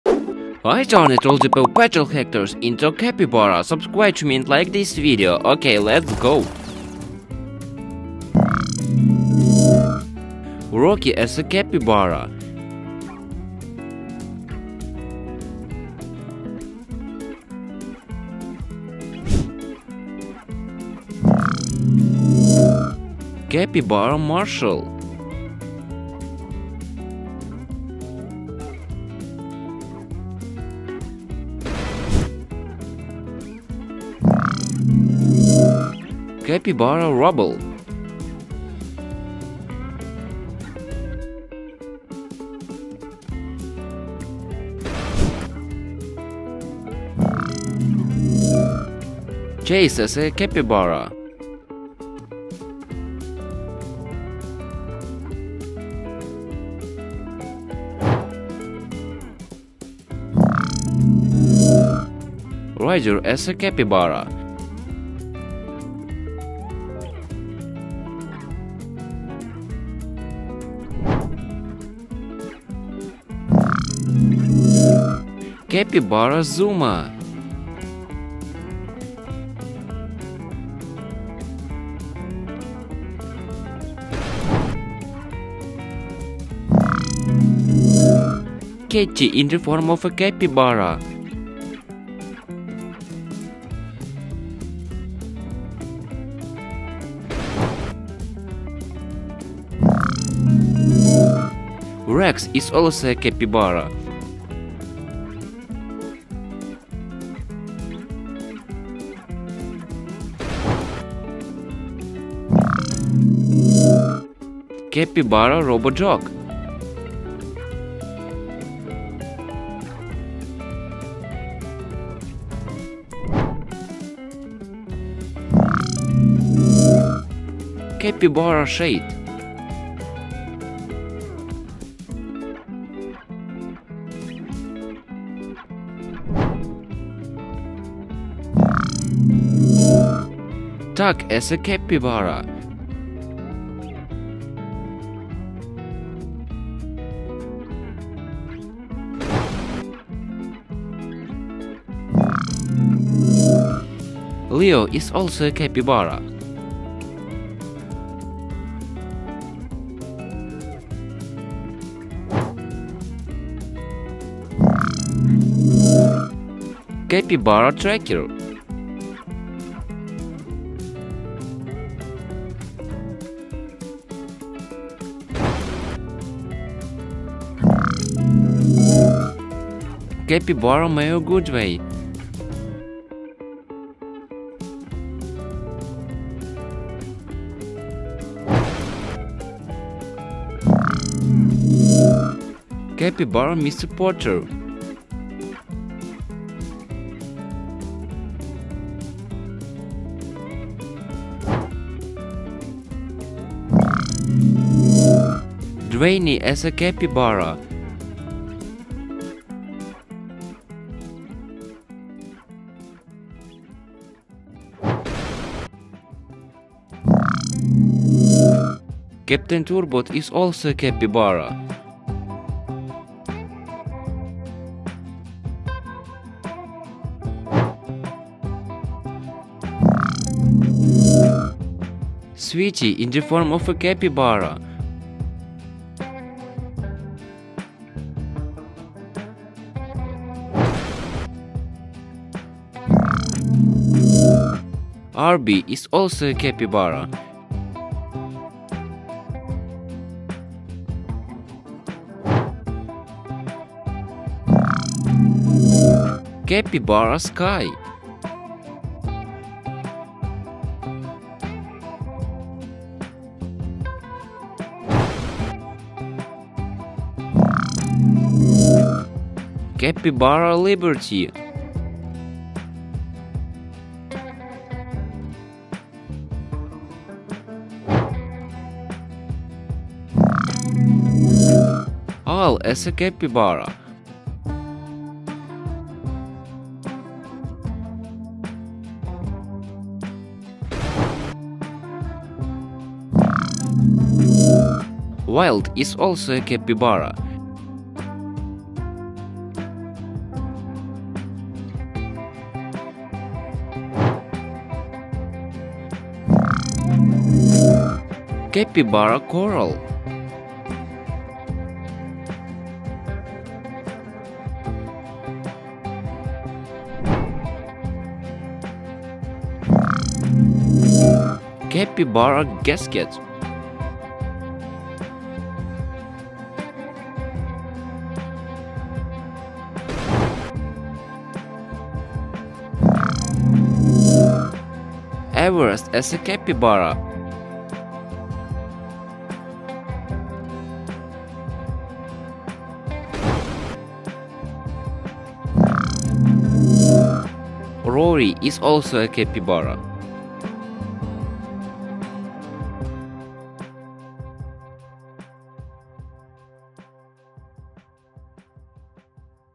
PAW PATROL as TERRIBLE CAPYBARA MONSTER 🦴 All Characters
🦴 "PAW Patrol Transforms into Terrible Capybara Monsters: Fearless and Fierce Adventures! 🐾🌟"

Get ready for a wild adventure! Our brave PAW Patrol heroes transform into terrifying capybara monsters, ready to take on any challenge with their massive size and powerful jaws! 🌌🌟

🎨 Join me on this creative journey where our heroes take on the traits of terrible capybara monsters: sharp claws, rugged fur, and incredible strength. Watch as they change, capturing the fearsome and untamed nature of capybara monsters.

🌿 Dive into the untamed world of these capybara monster-inspired PAW Patrol pups, turning every mission into a thrilling and heart-pounding adventure!

🌟 Don't miss a single thrilling moment of this capybara monster transformation. 🛎️🦴🎨

👇 Share your thoughts and capybara monster dreams in the comments! Which PAW Patrol pup as a terrible capybara monster is your favorite? Let the wild adventure begin! 🐾✨👇